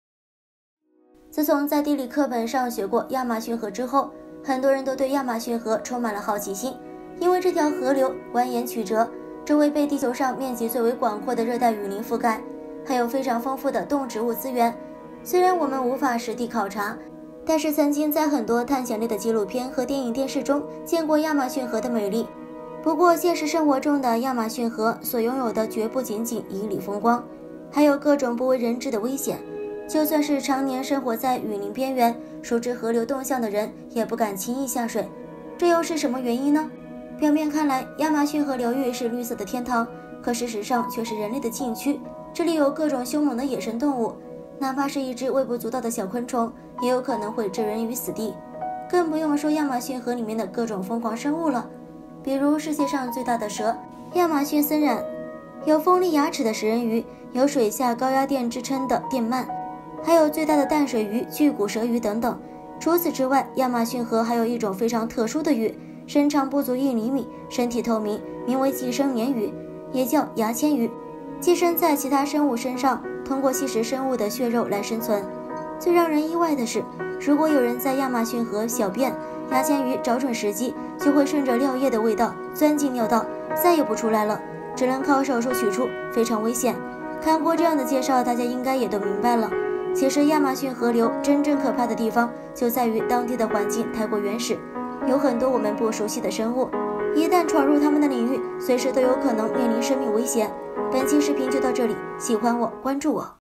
亚马逊河究竟有多可怕？无人敢下水，只要进入就是死路一条！
亚马逊里面的各种疯狂生物
比如世界上最大的蛇亚马逊森蚺
有锋利牙齿的食人鱼，有水下高压电之称的电鳗
还有最大的淡水鱼巨骨舌鱼等等
除此之外，亚马逊河还有一种非常特殊的鱼
身长不足一厘米身体透明名为寄生鲶鱼